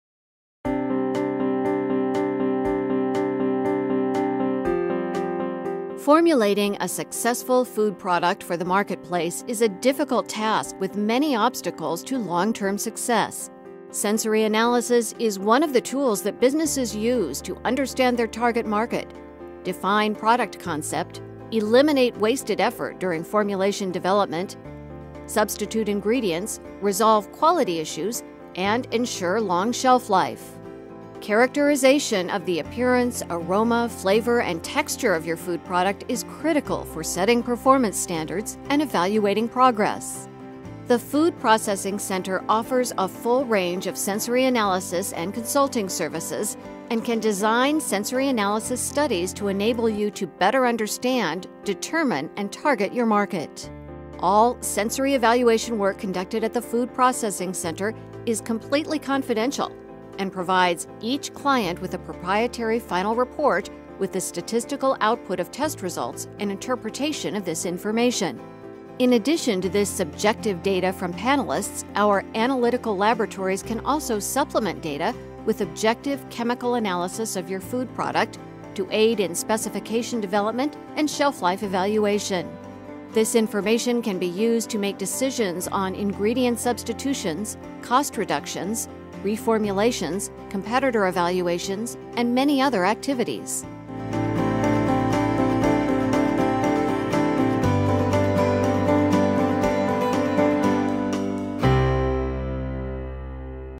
Food Processing Center: Sensory Lab
Without sensory analysis, your company may use valuable resources developing a product that misses the objective. Sensory evaluation is one of the tools that businesses use to understand their target market, define product concepts, streamline formulation development, substitute ingredients, resolve quality issues and ensure long shelf life. Characterization of the appearance, aroma, flavor and texture of your food product is critical for setting performance standards and evaluating progress.